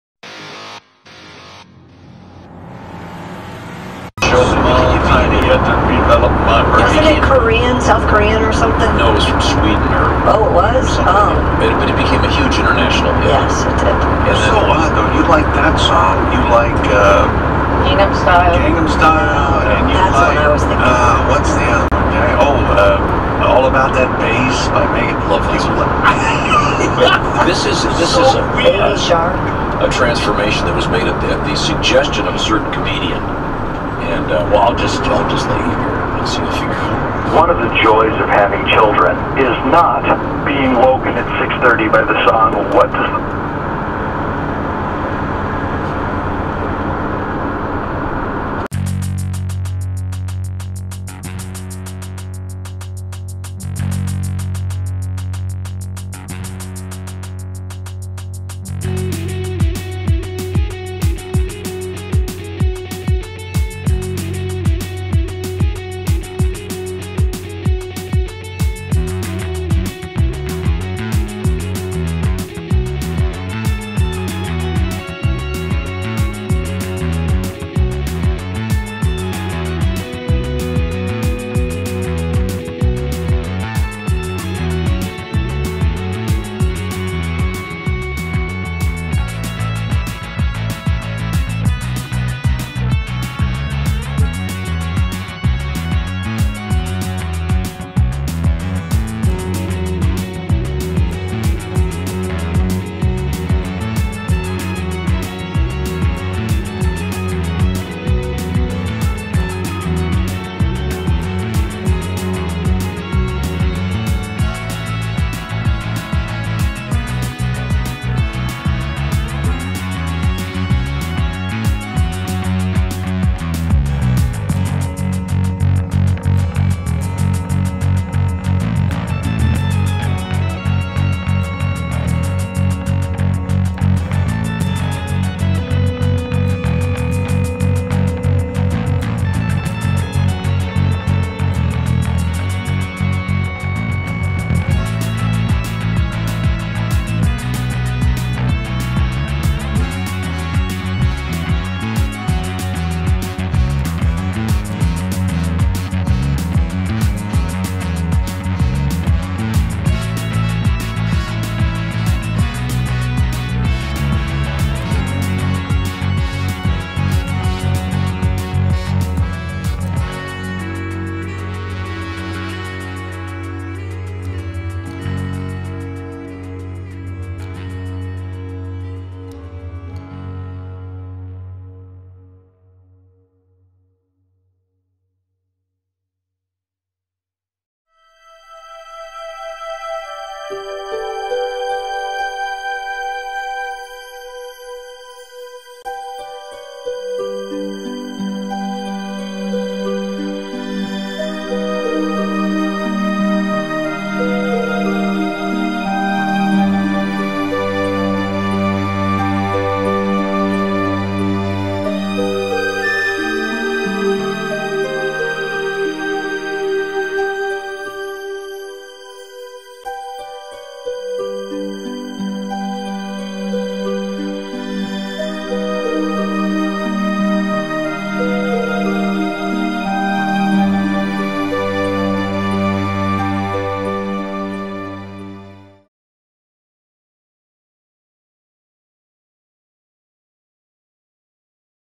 Trafficality 32: Go Go Gadget Rockstar
Song by The Whole Other

The dashcam clips are free use. You may download, edit, upload to the forum of your choice for general discussion and\or bad mouthing.